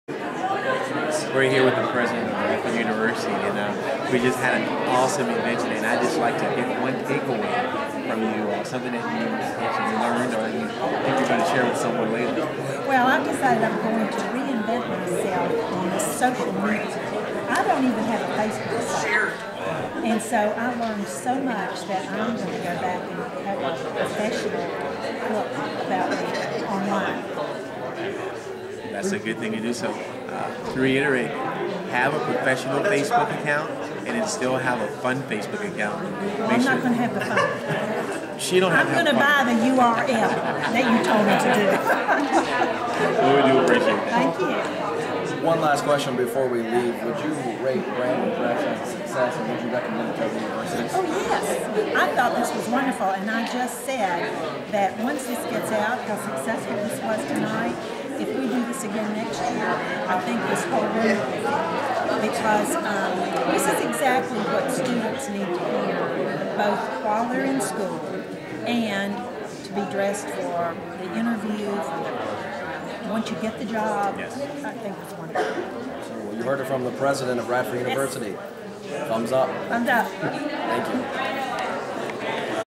THE IMAGE DFS Runway Show Reference from Dean at Radford University
What are professional, business casual and smart casual dress?  Speaker and Image Coach Bron Austin Deal addresses young professionals on the essentials of corporate image and how to get there.